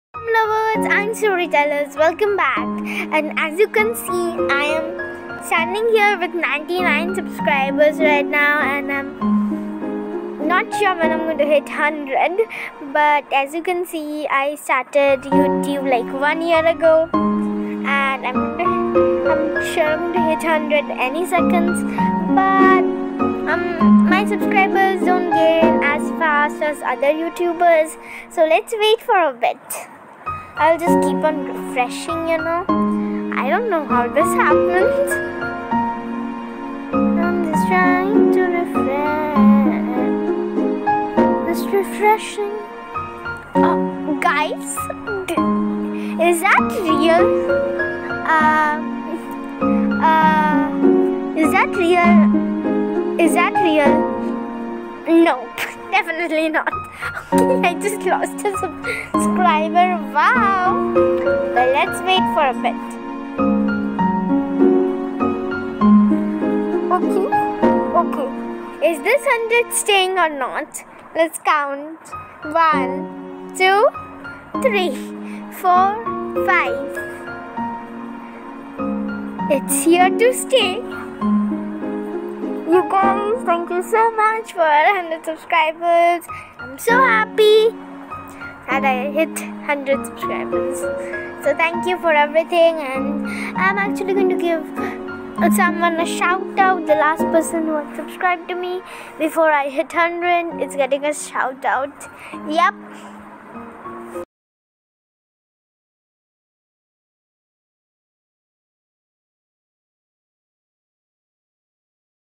Yay!!! Thank you for 100 subs | Ahana Quazi
hi poem lovers and storytellers welcome back and today I reached 100 subscribers and this proves that nothing is impossible and you should believe in your self it took me 1 year to get 100 subs and I am sooo happy and you should know that if the path to your goal is Rocky then change the path but never the goal ! byeee hope you have a great day
Stay Safe!